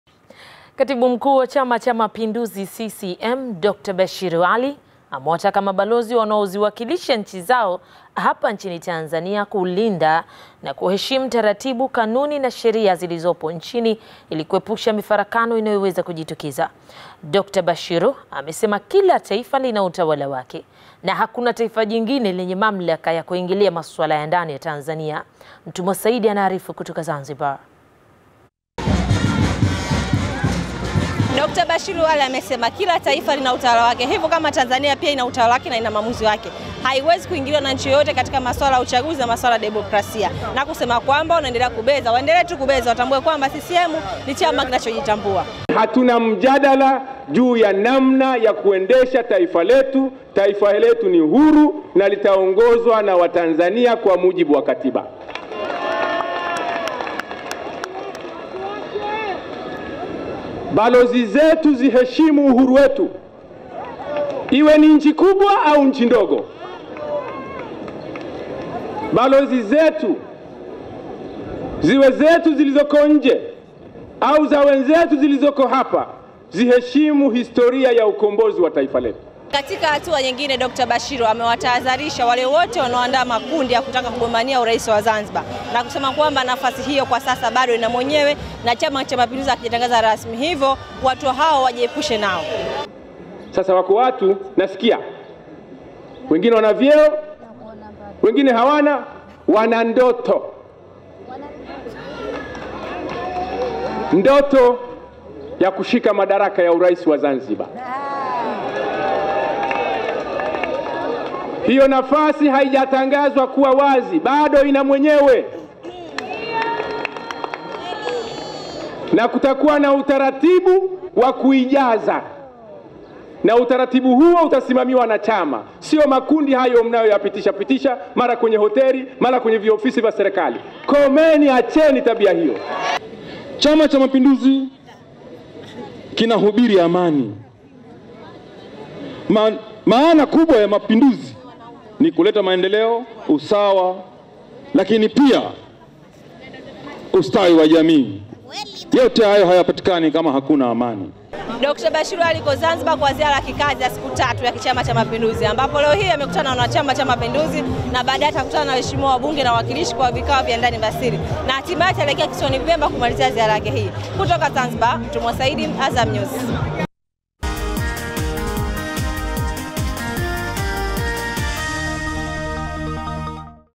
Katibu Mkuu CCM Dkt. Bashiru Ally awajia juu Mabalozi, asema Tanzania ni huru
“Taifa letu ni huru na litaongozwa kwa mujibu wa sharia, Balozi ziheshimu uhuru wetu…hakuna taifa jingine lenye mamlaka ya kuingilia masuala ya ndani ya Tanzania,” ni kauli ya Katibu Mkuu wa Chama Cha Mapinduzi CCM Dokta Bashiru Ally.